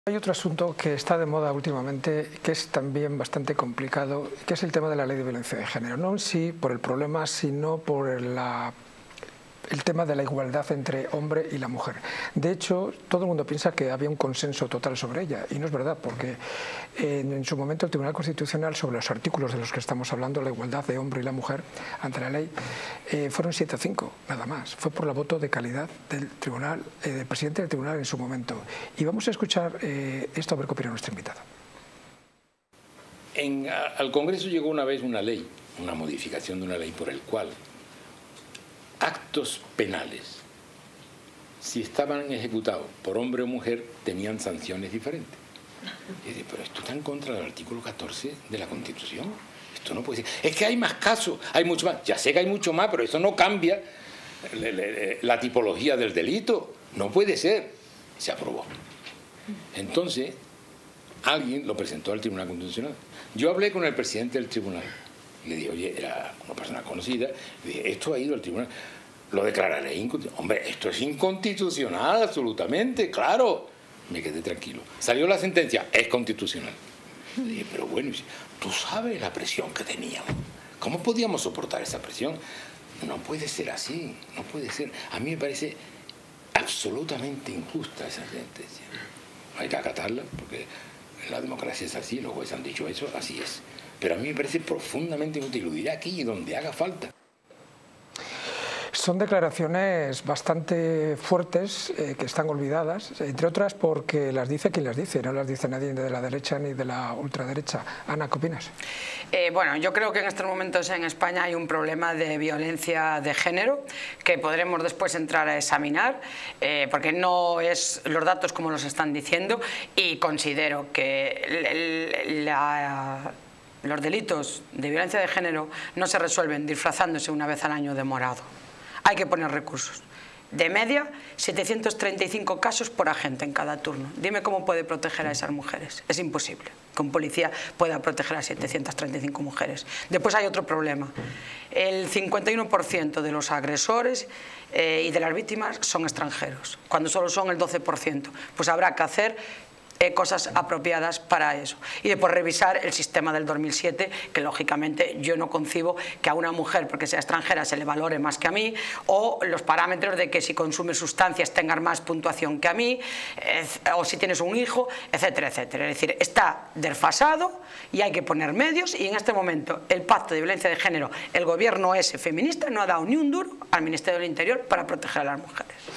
🔴ANA BELÉN VÁZQUEZ, 51% DE LOS AGRESORES DE VIOLENCIA DE GÉNERO SON EXTRANJEROS🔴
En esta entrevista, Ana Belén Vázquez discute las estadísticas sobre la violencia de género, destacando que el 51% de los agresores son extranjeros. ¡No te lo pierdas!